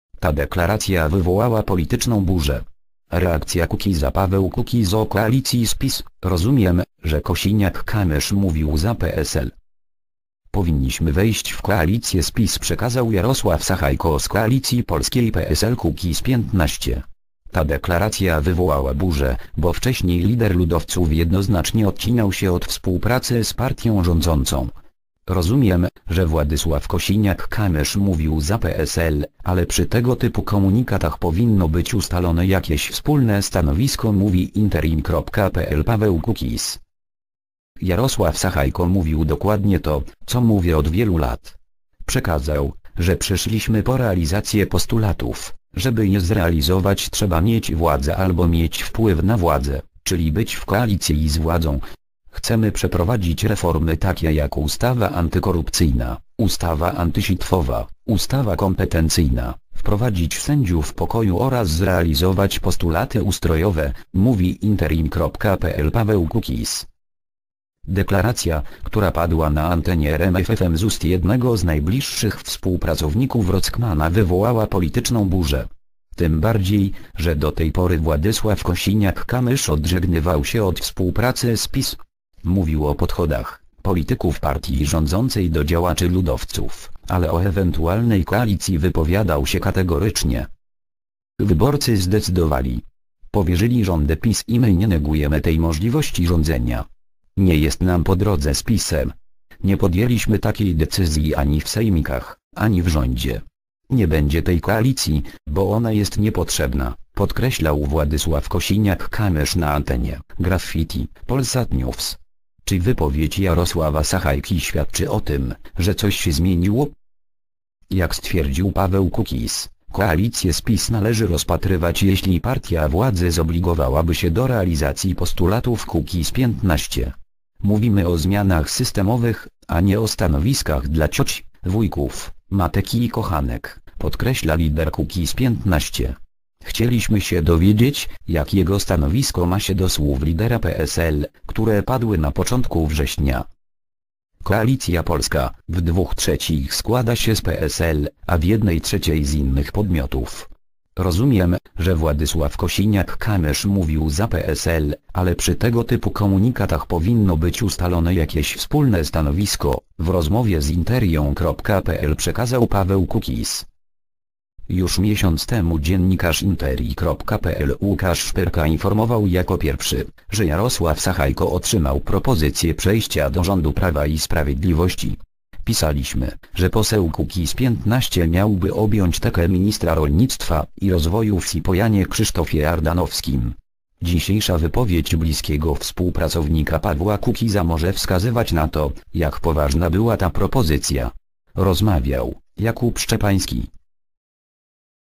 Ta deklaracja wywołała polityczną burzę. Reakcja Kukiza.
Paweł Kukiz o koalicji z PiS: Rozumiem, że Kosiniak-Kamysz mówił za PSL.